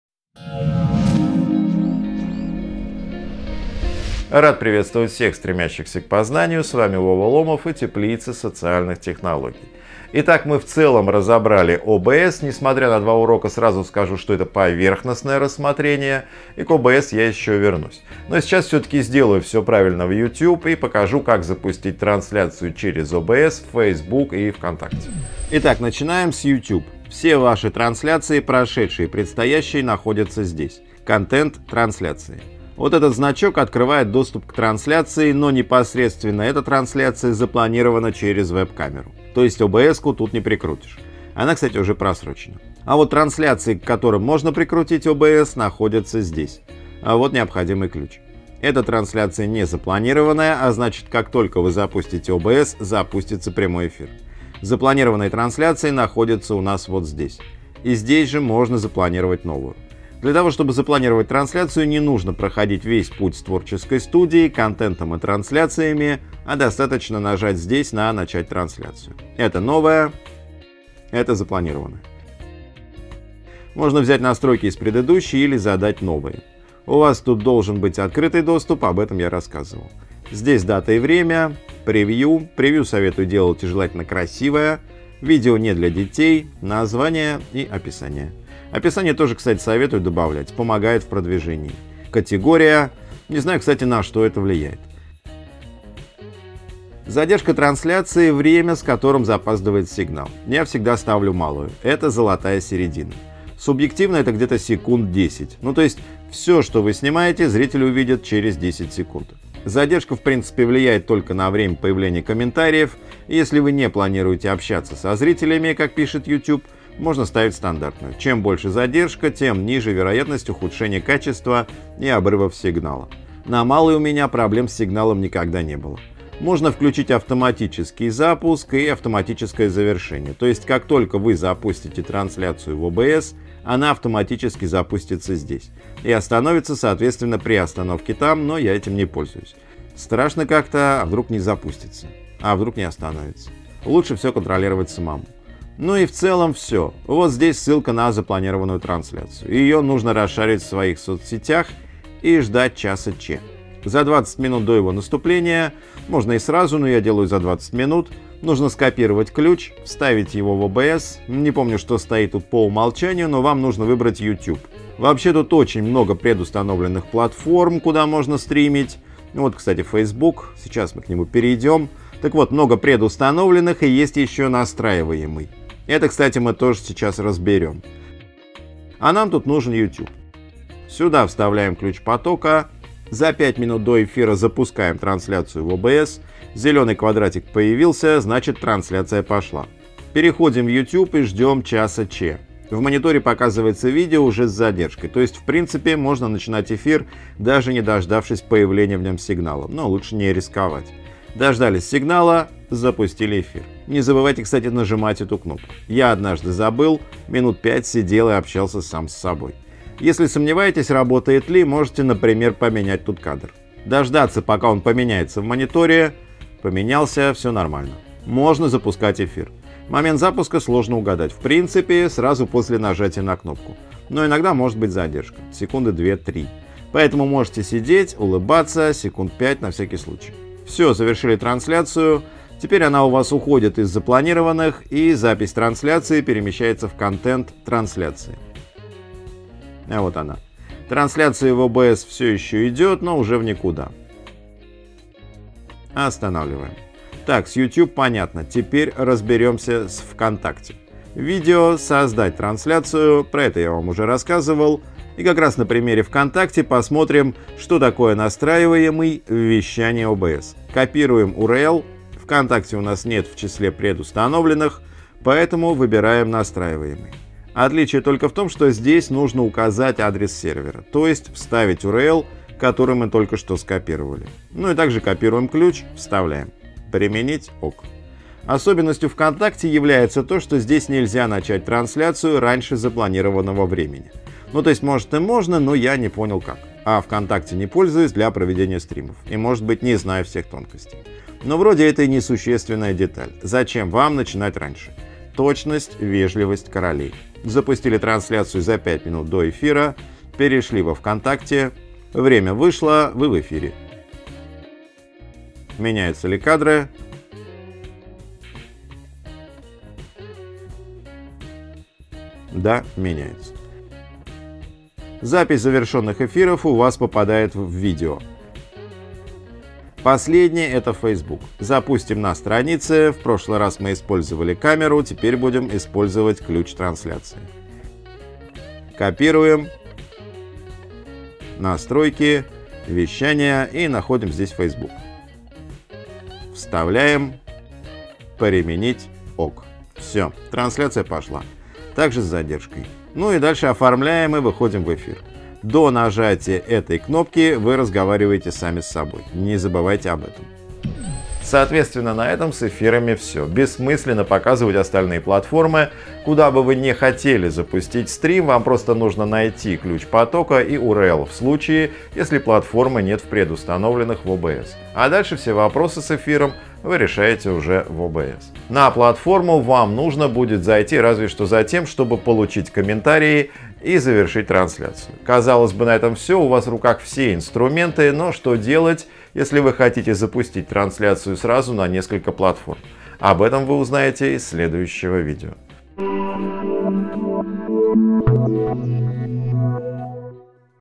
Как сделать стрим #5: стрим в YouTube, Facebook и VK через OBS
Провести стримы в YouTube, Facebook, Вконтакте можно без особой подготовки. И кажется, что поэтому все уже их ведут или пробовали вести хотя бы раз. Но на самом деле, всегда остаются те, кто не знают как это делать. Или знают, но раньше не задумывались о том, как провести стрим качественно. В новом курсе Теплицы социальных технологий Вова Ломов расскажет о том, как делать стримы просто и качественно.

Начнем с обычной вебкамеры и прямого эфира без подготовки и дополнительных объектов в кадре. И затем перейдем к OBS Studio и сложным эфирам с большим количеством сцен. Разберем все нюансы работы с OBS Studio и закончим обзором Blackmagic Atem Mini, с помощью которой можно микшировать до четырех входных каналов видео. А четырех каналов с хорошими камерами и операторами достаточно, чтобы вывести в эфир концерт группы Portishead.